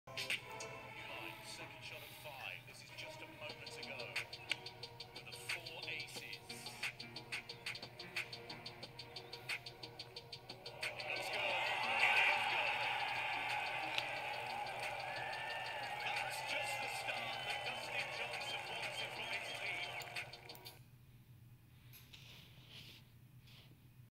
Peter W AND"Dusty Johnson's Playoff Advantage 4 Aces in 18 Seconds!" getmunch Advantage Four Ac
Dusty Johnson's team just pulled off 4 aces in a row in the playoffs - a huge advantage! You can't beat the skill and determination of DJ and his crew!